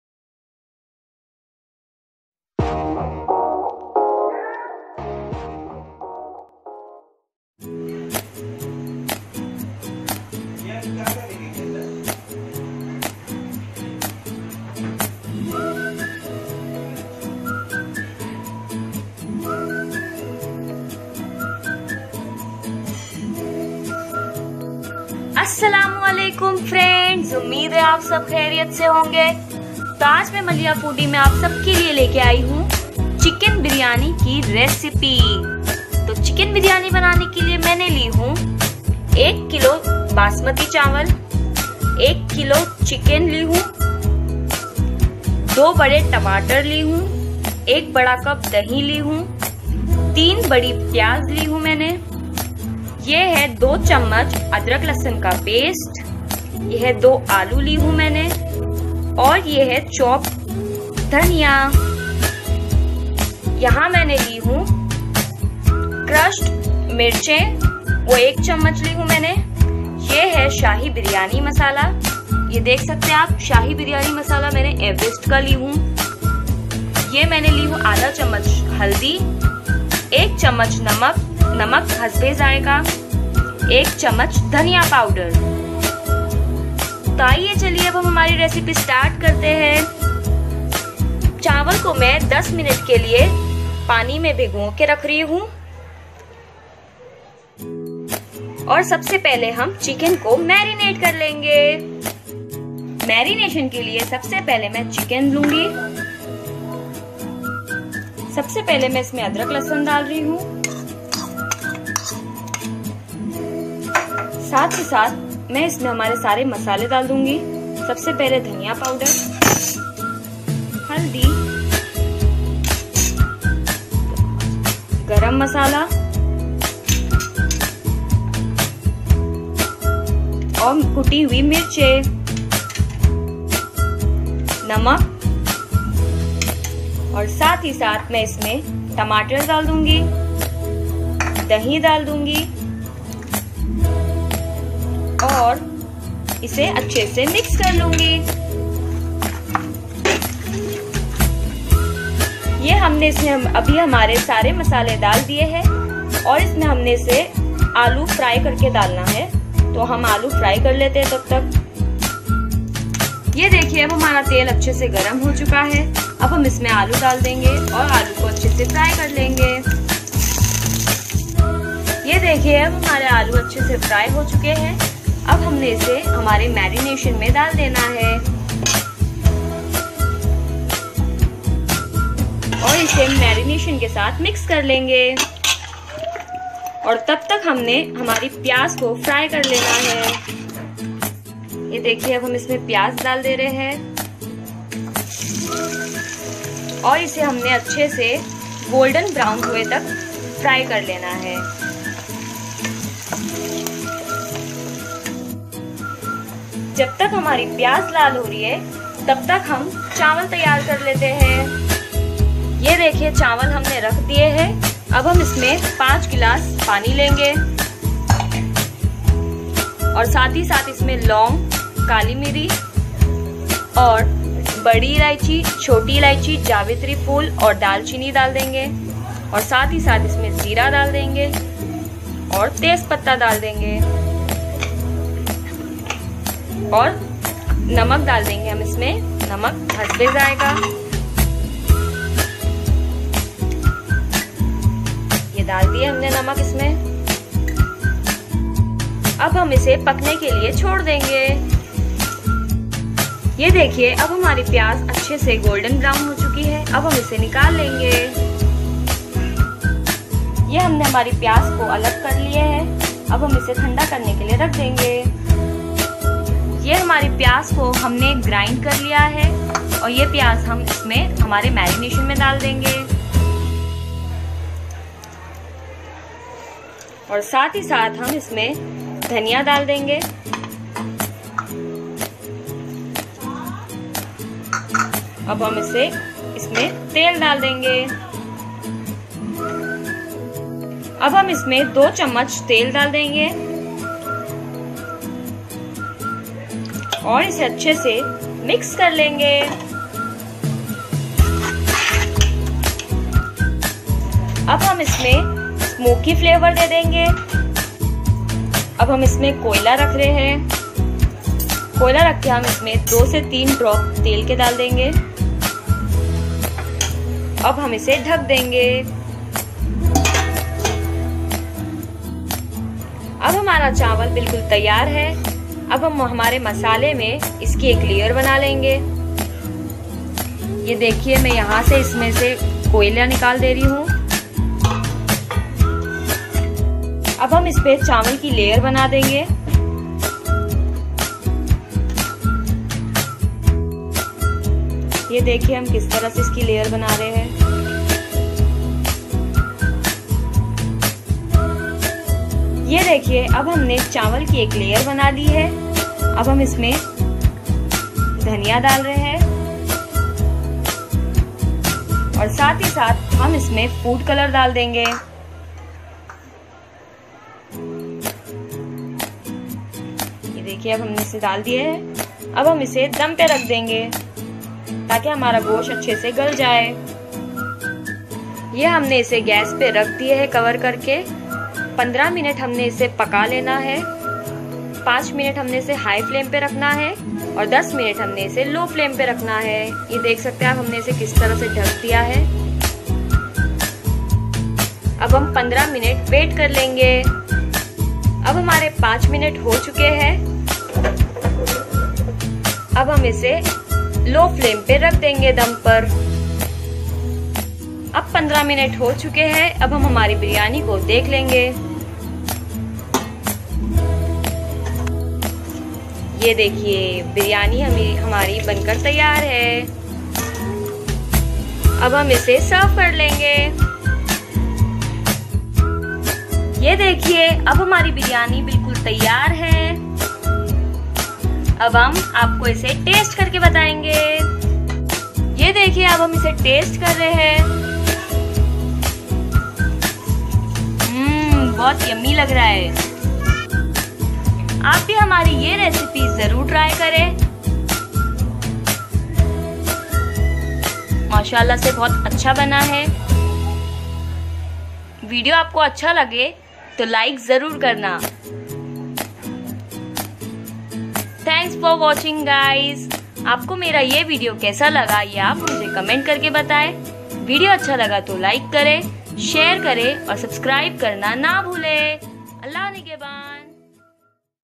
Kache Gosh ki Chicken Dum Biryani | By Maliha Foodie
Ingredients:
1kg - Basmati Rice
1kg - Chicken
2 - Tomato
1 Cup - Curd
3 Onion
2 Spoon - Ginger garlic paste
2 Potatoes
Chopped Coriander Leaves
1 Spoon - Crushed Green Chillies
1 Spoon - Biryani Masala
Half Spoon - Turmeric powder
Salt as per taste
1 Spoon - Coriander Powder